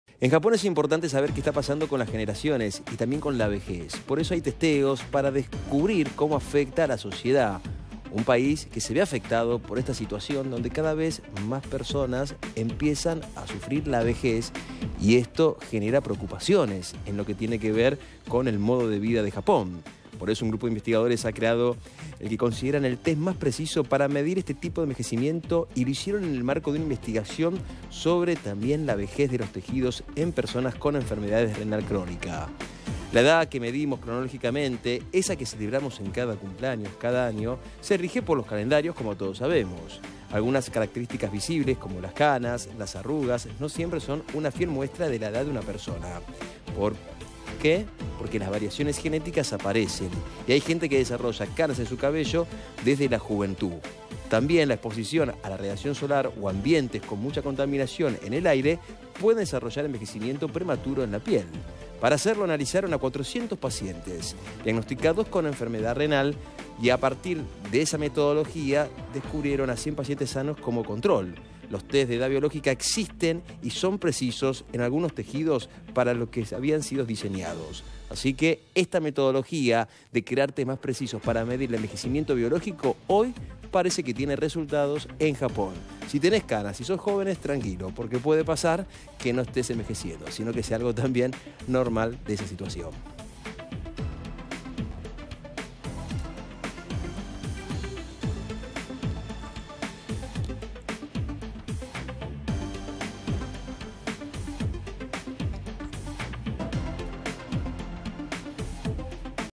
Crean test para medir el envejecimiento biológico
👉 Un grupo de investigadores ha creado el que consideran el test “más preciso” para medir este tipo de envejecimiento y lo hicieron en el marco de una investigación sobre el envejecimiento de los tejidos en personas con enfermedad renal crónica.